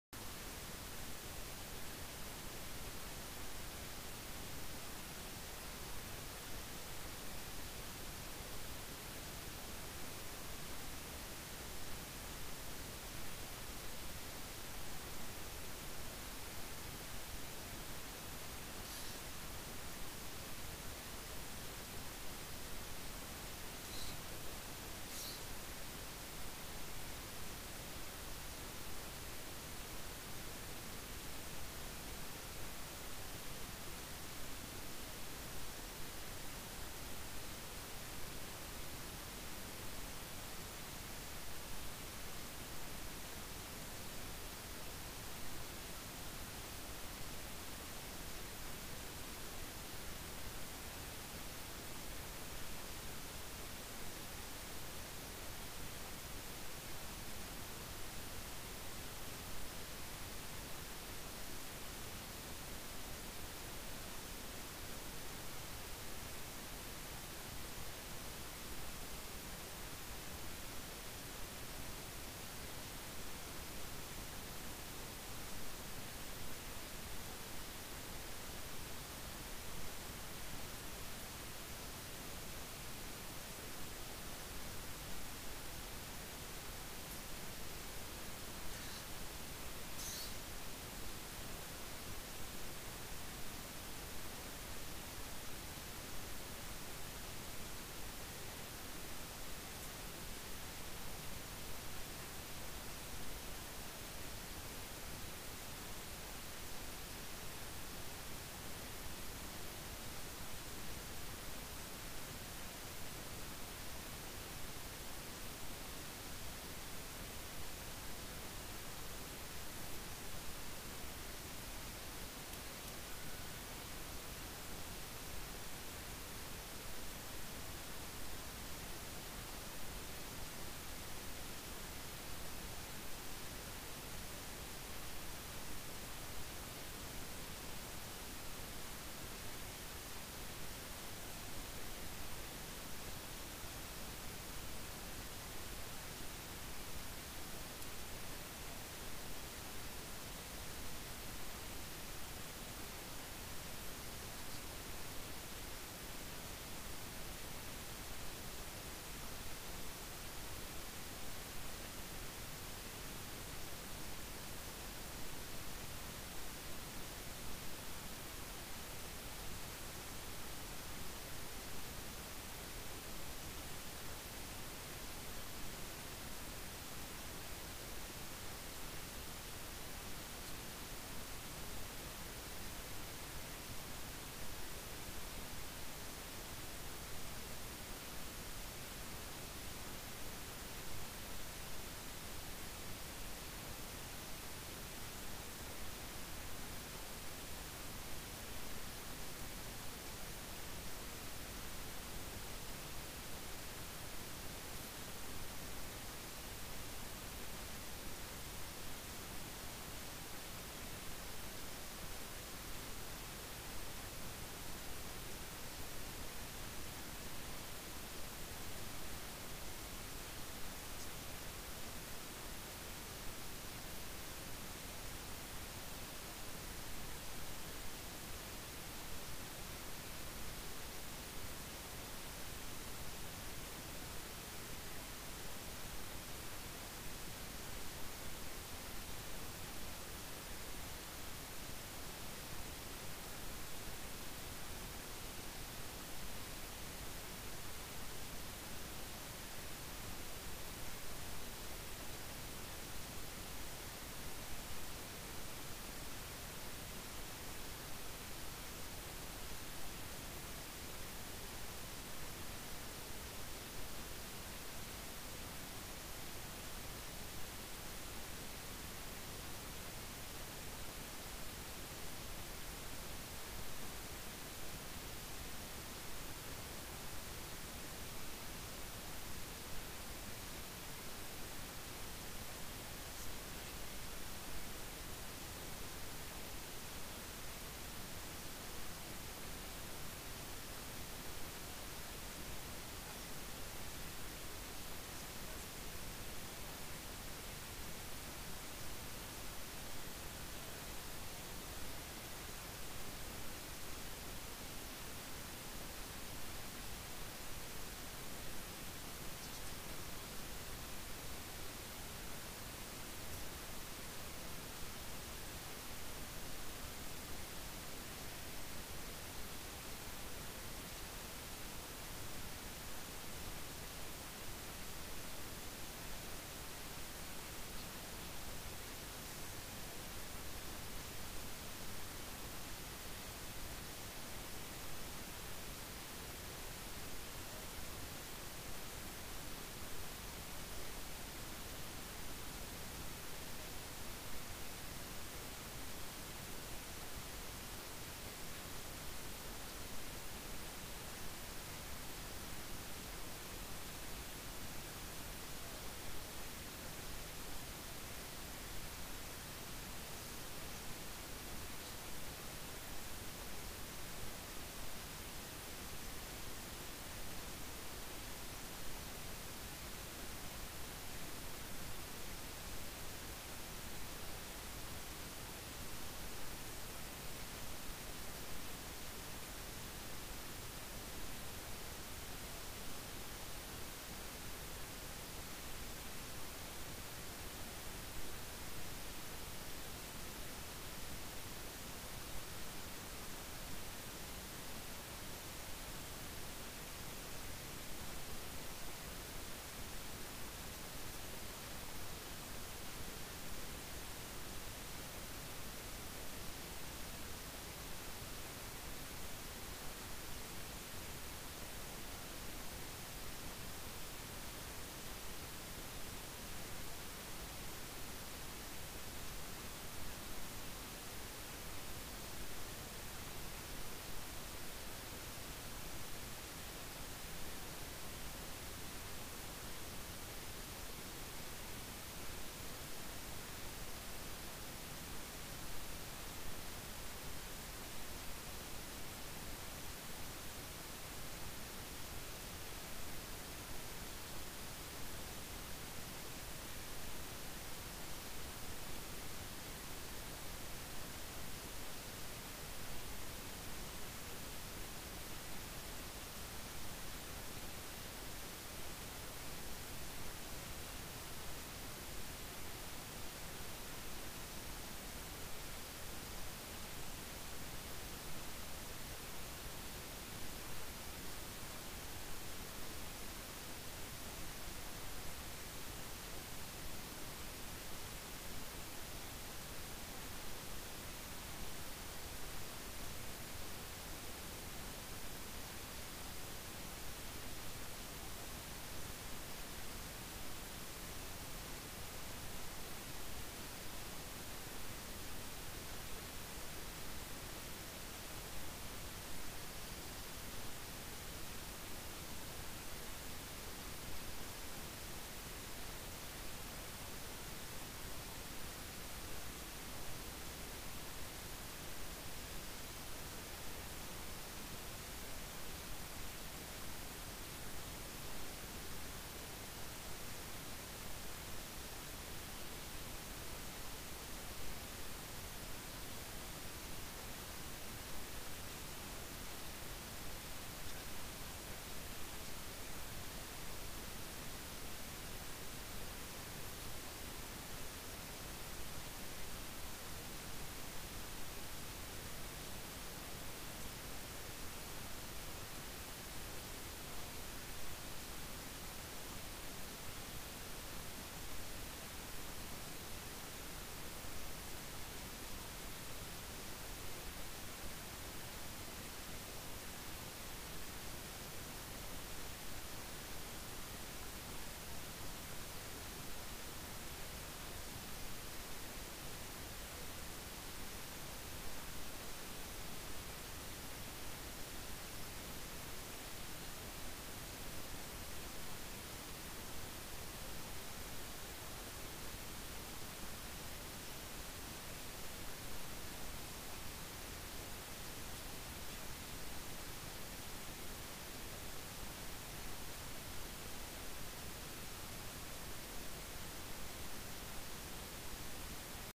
Lions Pt.3
11/09/07 Djuma Game Reserve
Sorry for no sound. For some reason it didn't record the audio.:(